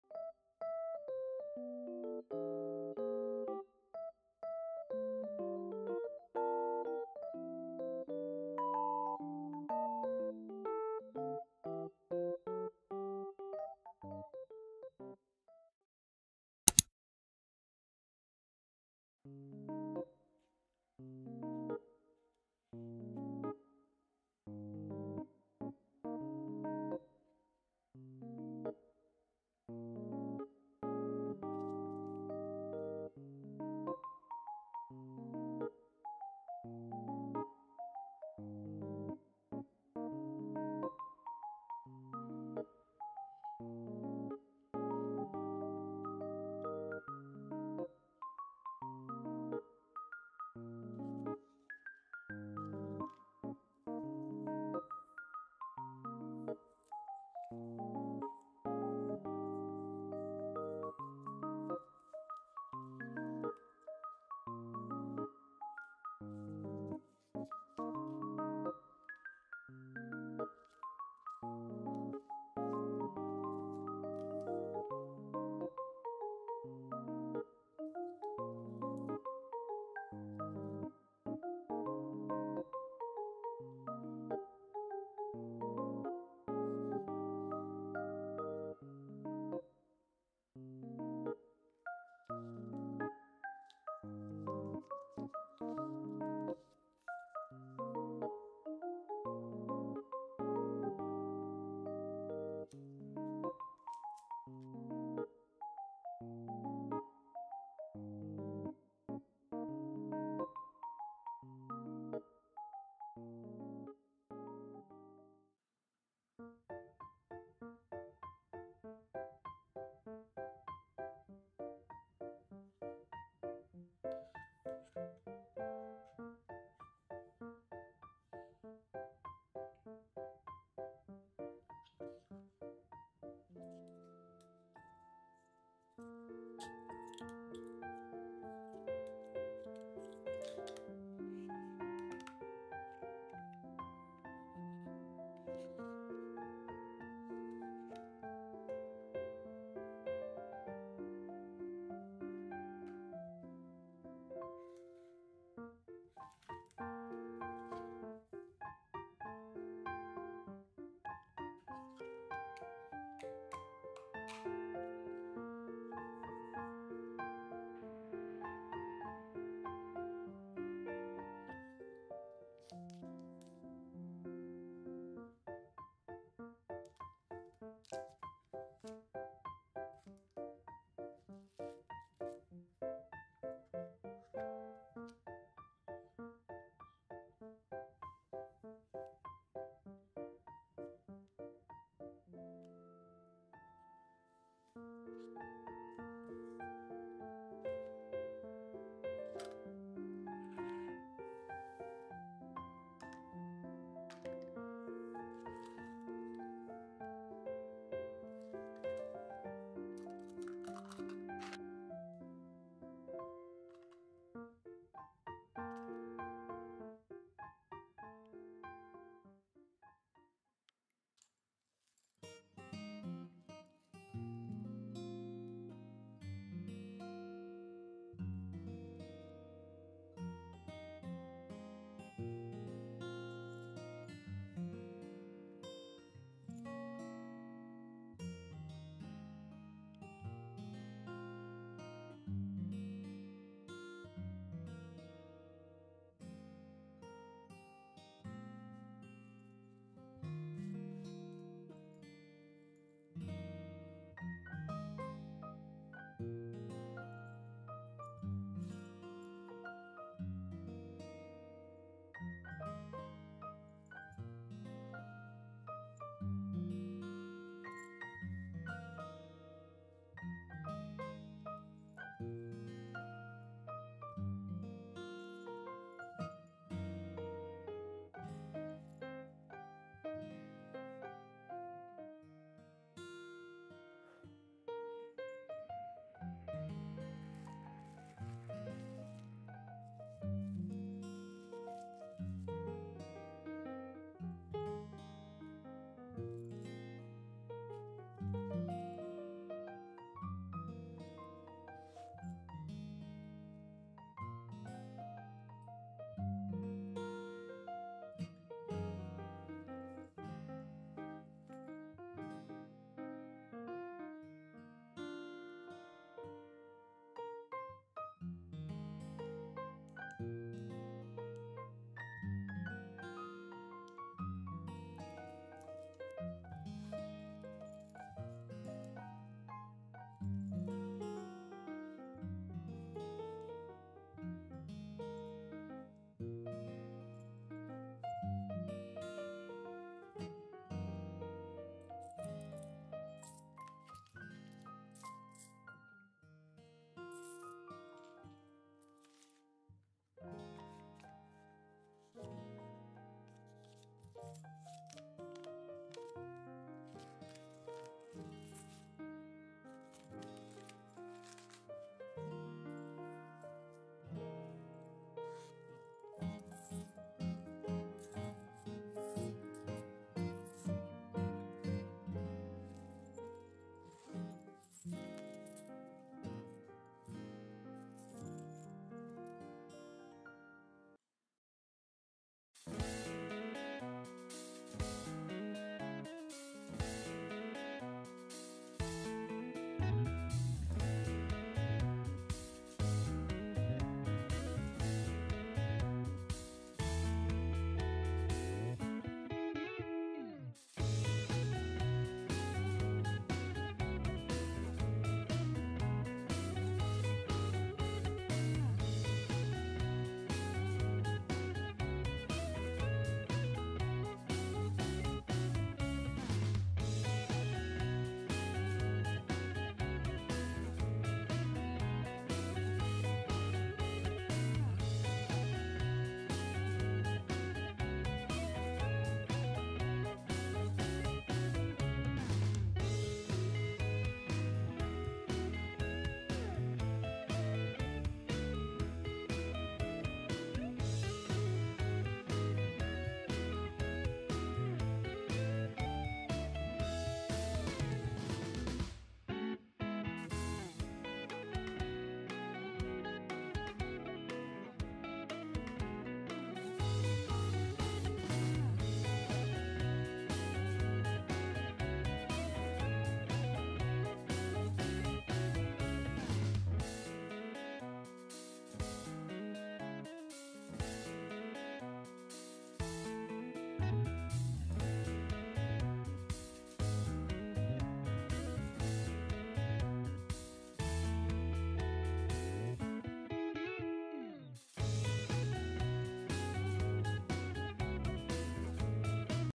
【再販】便利過ぎて即完売のアイテムが戻ってきた！
セリア購入品／カード保護スリーブ／デスク小物立て/Wファスナーポーチ(ジェントルマン)